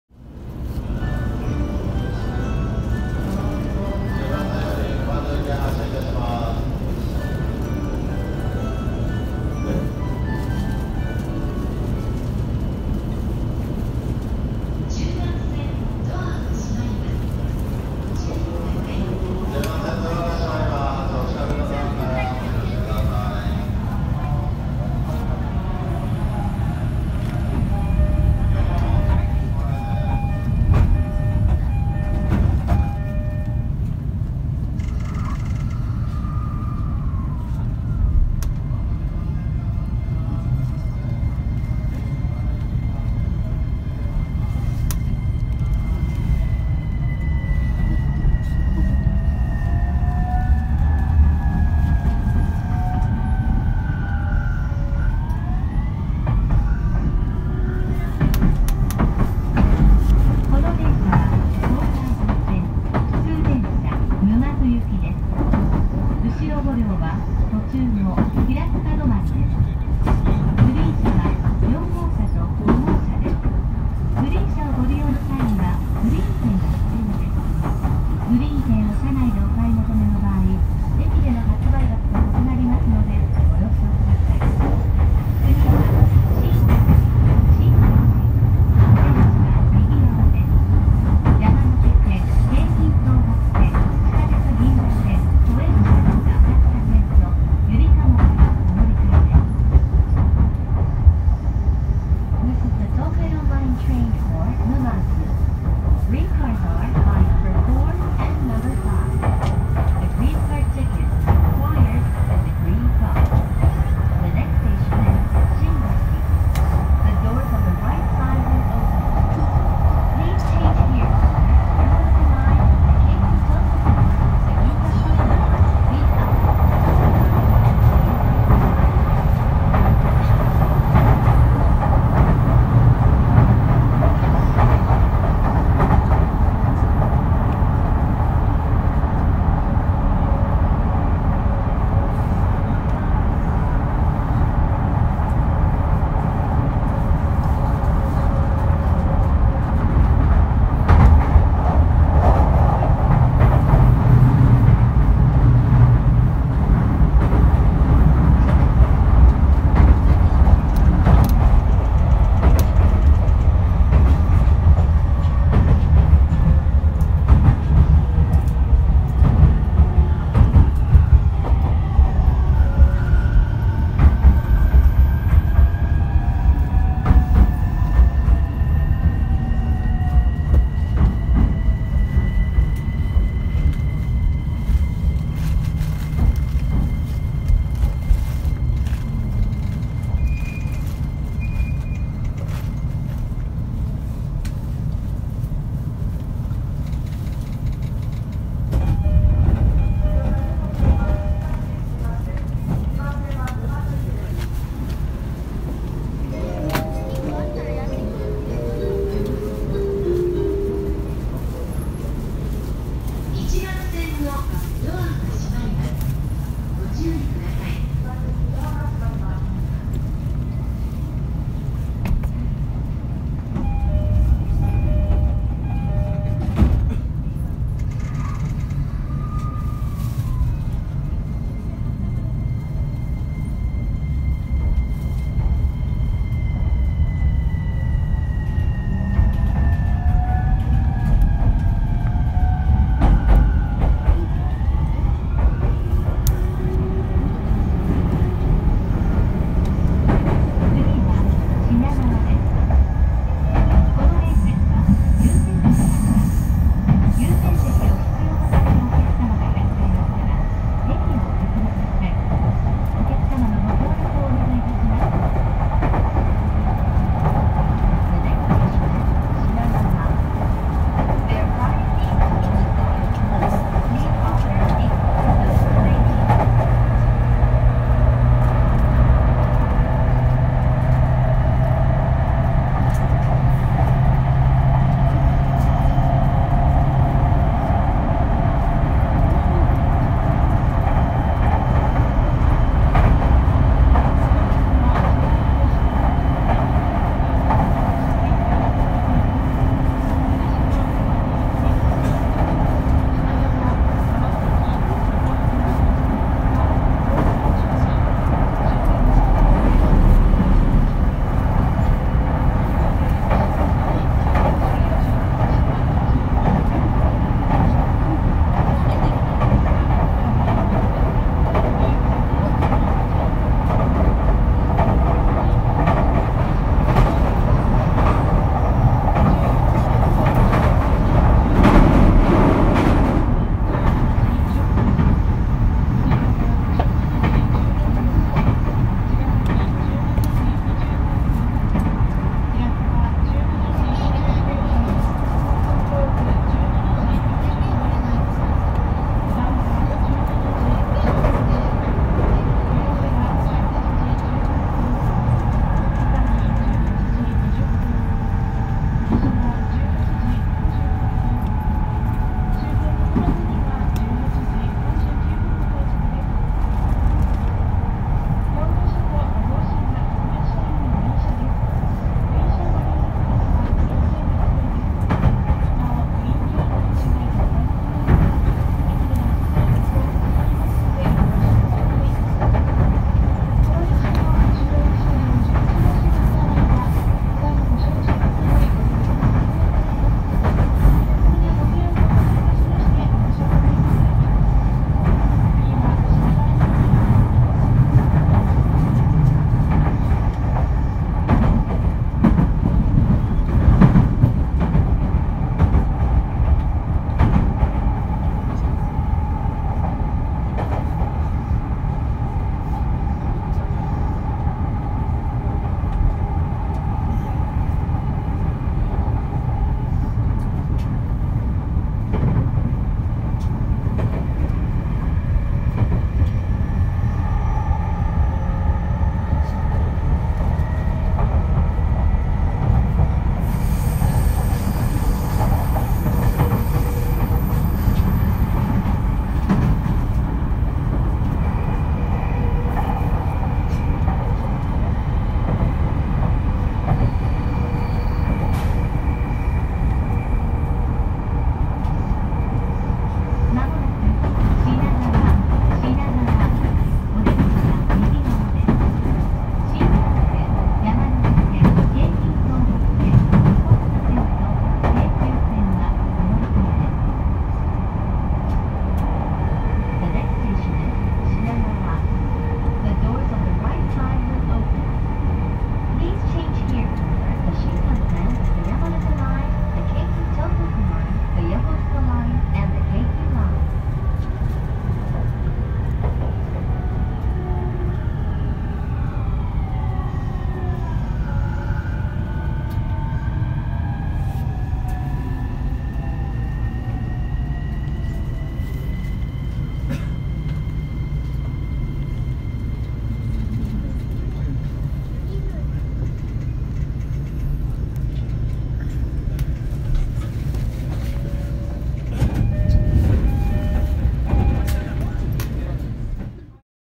（１２）【走行音】E231系東海道線 東京→品川 墜落インバーター 2018.04
この動画は東海道線（上野東京ライン）の小山〜横浜駅間の全14回の動画の中の1つです